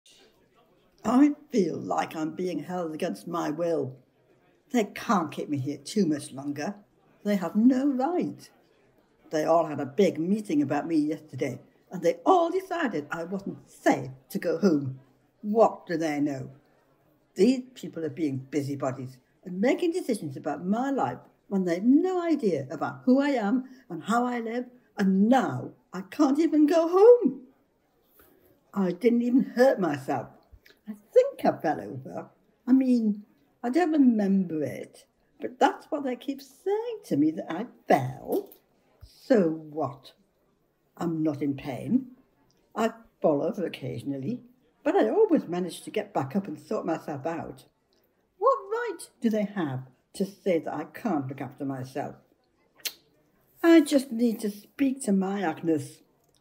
Frailty Patient 360 Experience
The following video simulation depicts a fictional scenario of a patient who lives with frailty. Move your screen around to see all around you and Sound On. At any time please remove your headset or break from the video. It has been taken from the Care for Older People virtual reality eLearning programme currently in development with the Northumbria Health and Care Academy.